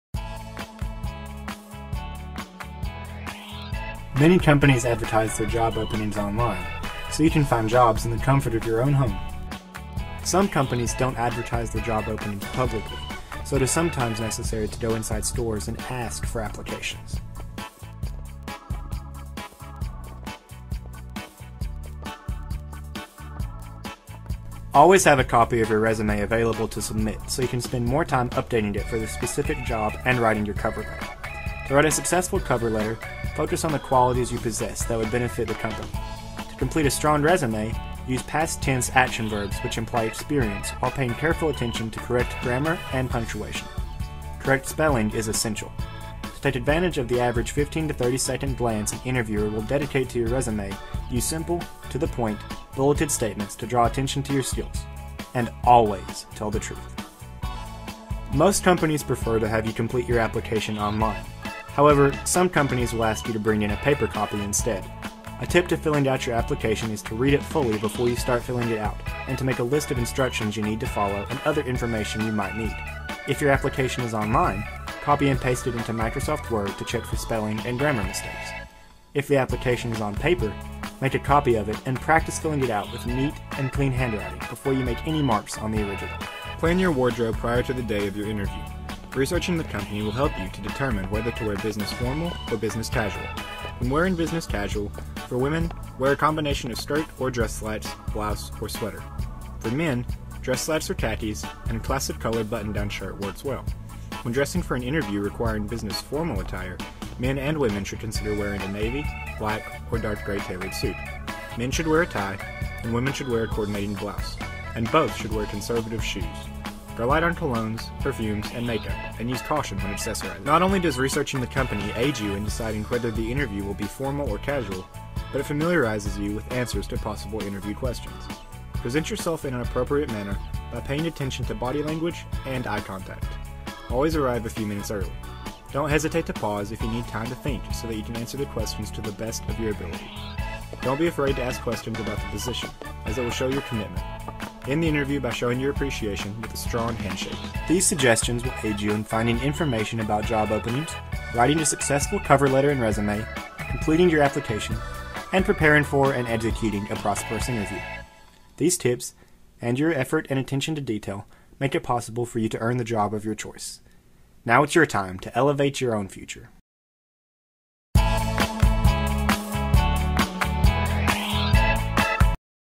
2018 3-D Animation, 5th Place
Topic: Using 3-D animation, create an informational video on how to get a job. At minimum, the video must include information on the following components: Where to find information on job openings; Writing a resume and cover letter; Completing application forms.

Students: Brooke Banner

State: Virginia

Software Used:
Moviestorm
OpenShot Video Editor
Sound Editor
M-Audio Microtrack II
Paint 2016
PowerPoint 2016

Copyright Notations: The fair use guidelines for educational purposes, provided in the Future Business Leaders of American Style Guide, were followed in selecting images and other information to use for this project.

Template(s) Used (source): No template was used.